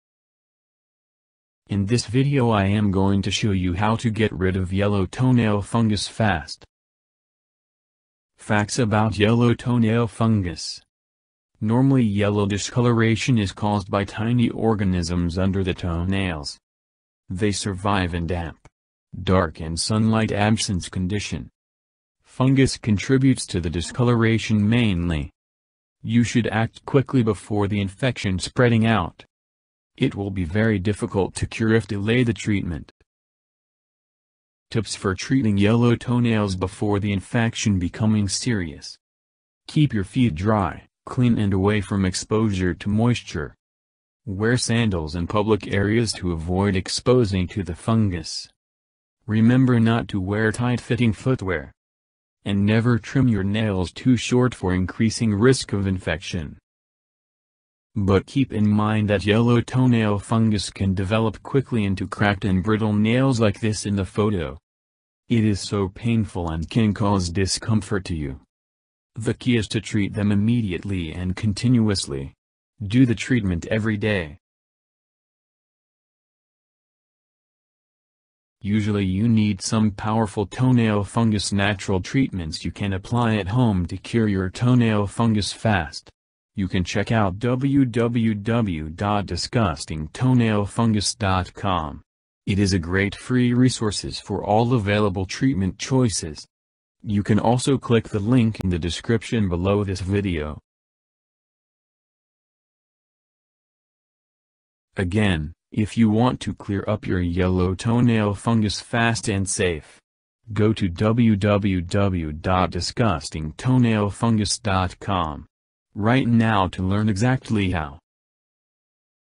How To Get Rid Of Yellow Toenail Fungus FAST("The Complete Guide")
for much POWERFUL and SAFE toenail fungus natural treatment that can cure your fungus FAST at home.

How To Get Rid Of Yellow Toenail Fungus FAST(The Ultimate Guide)

Yellow discoloration is caused by some tiny organisms under the toenails. They normally can survive in damp, dark and sunlight absence condition. Fungus contributes to the discoloration mainly.

When you suspect toenail fungus you should act quickly before the infection spreading out. It is very difficult to cure if you delay the treatment. So how to treat the yellow toenail before the infection spreading out?

1. You should keep your feet dry, clean and away from exposure to moisture.
2. Wear sandals in public areas to avoid exposing to the fungus.
3. Remember not to wear tight fitting footwear.
4. Never trim your nails too short for increasing risk of infection.

But yellow toenail fungus can develop quickly into cracked and brittle nails which can is very painful and can cause discomfort to you. So the key is to treat them immediately and continuously. Do the treatment everyday.

If you want to get the most effective, fast and safe natural toenail fungus treatments, then go